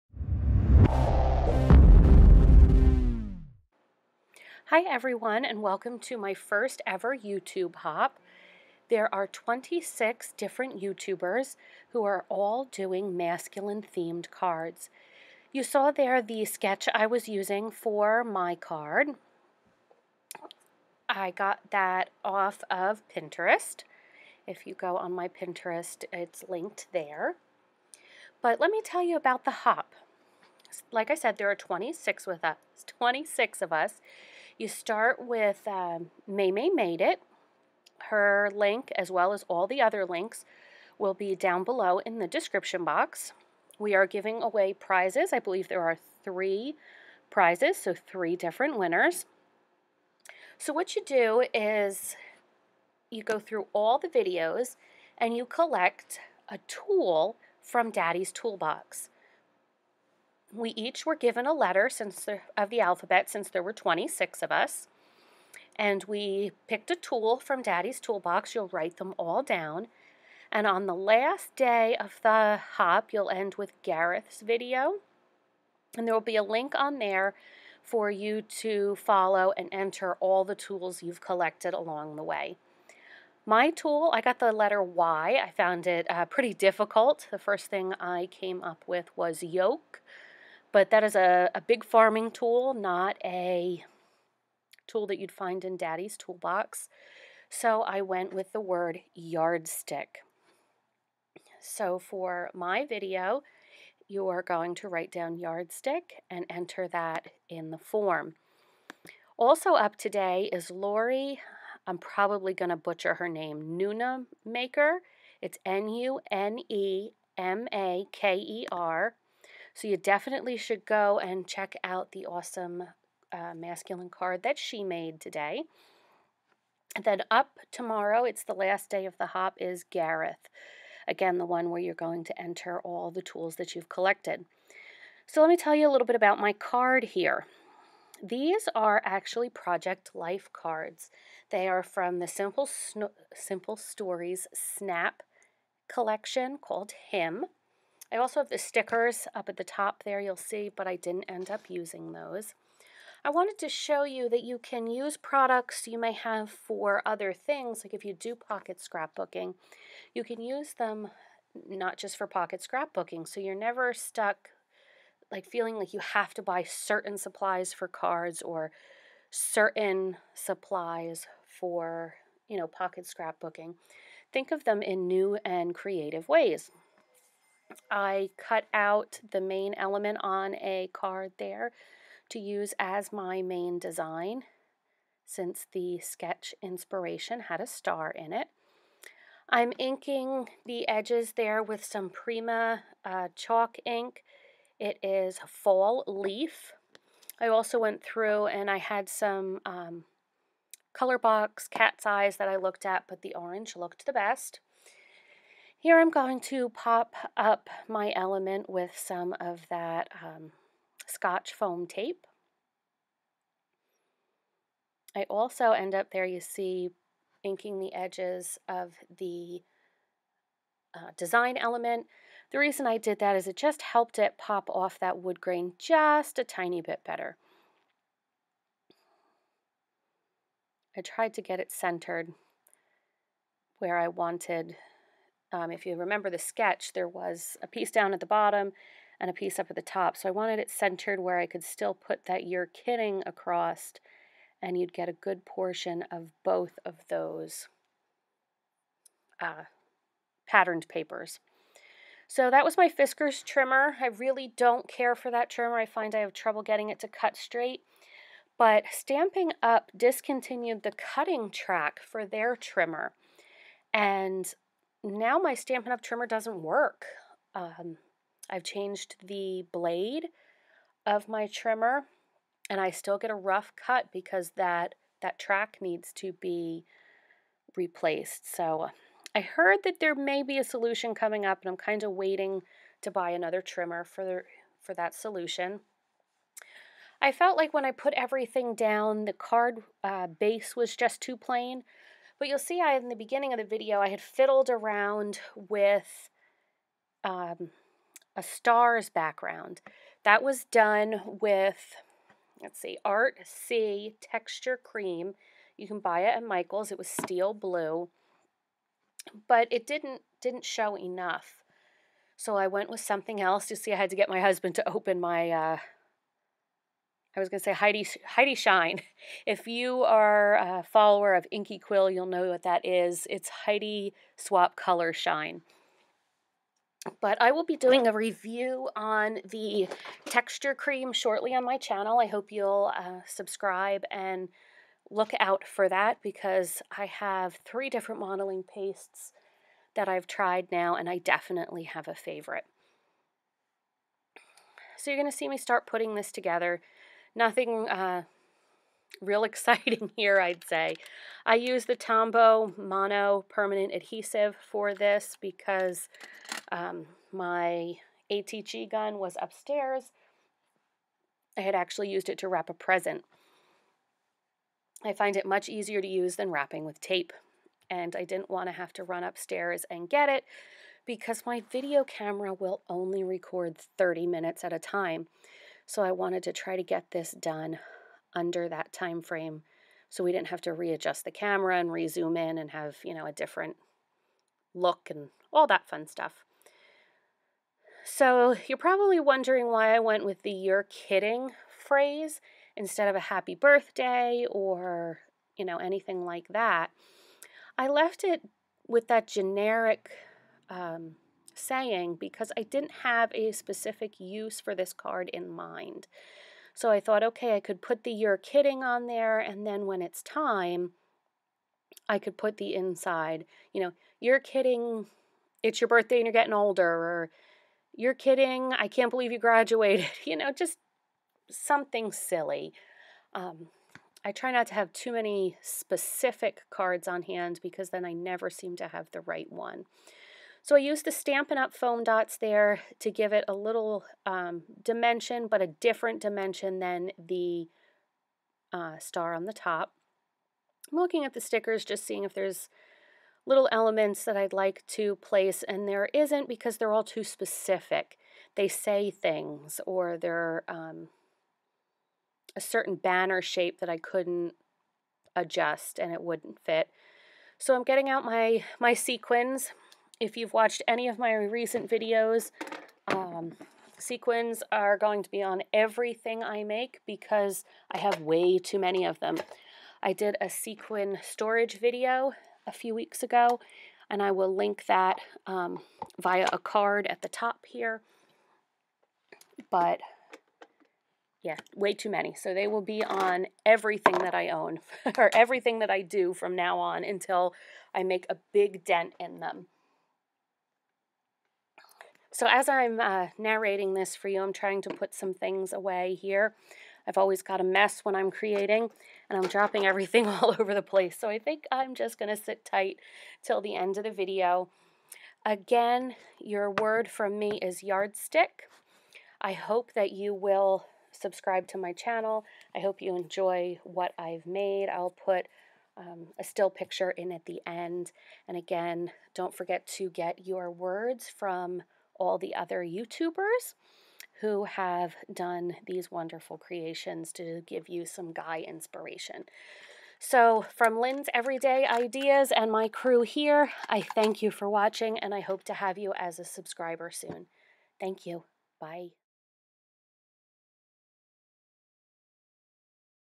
Guy Card Collaboration Video Hop Featuring Simple Stories
We have put together quite the group of YouTubers to hopefully inspire your Guy themed cards! Just in time for Father's Day 2016 and perfect for the men in your life.

There will be26 videos starting May 23rd through June 5th. We will do this hop style, so you will watch the first video on Maymay Made It's Channel then you will hop to the others using the description box to see who is next.

The contest is super fun, each creator will name a tool from their dad's tool box using the alphabet to keep us straight. You can start a list on your paper A thru Z and each time you catch a video write down the tool mentioned in the video. In the last video (Gareth's) there will be a link to a giveaway, you will list the tools you collected to be entered. Open internationally to persons 18 years and over.

And

And